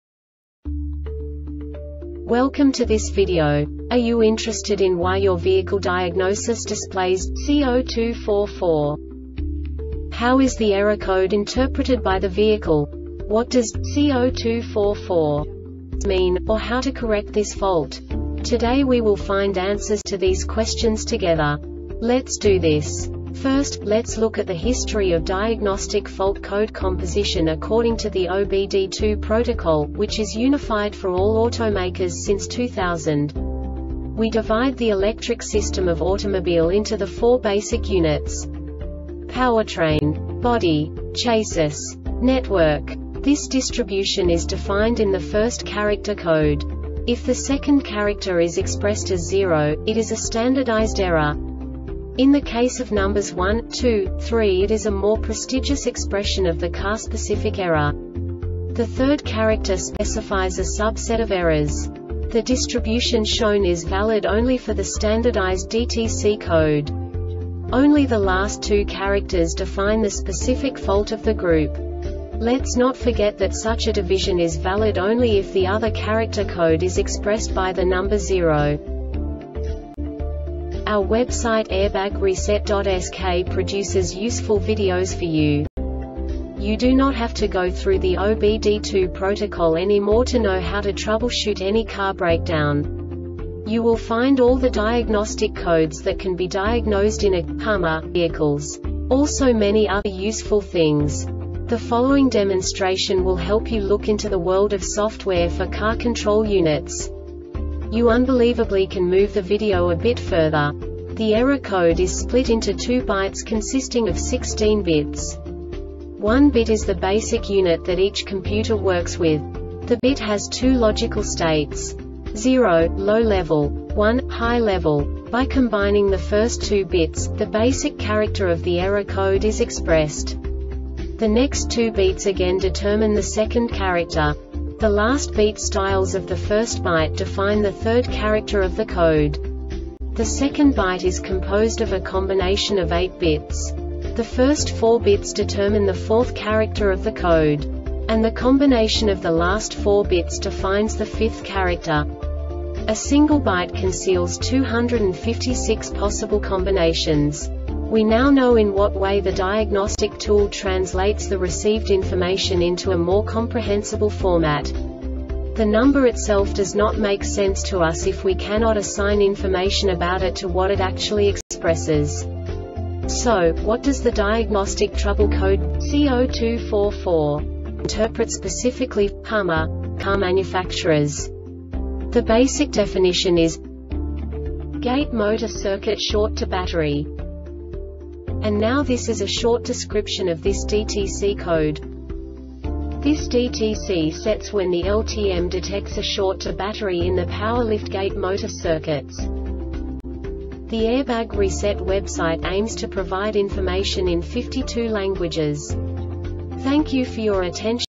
DTC Hummer C0244 Short Explanation
Contents:
0:21 Basic DTC analysis according to OBD2 protocol standard.
1:48 Insight into programming
2:58 A diagnostically understandable description of the Diagnostic Trouble Code
3:05 Basic error definition

Make:
HUMMER
Code:
C0244
Definition:
Gate Motor Circuit Short to Battery
Cause:
This DTC sets when the LTM detects a short to battery in the power liftgate motor circuit(s).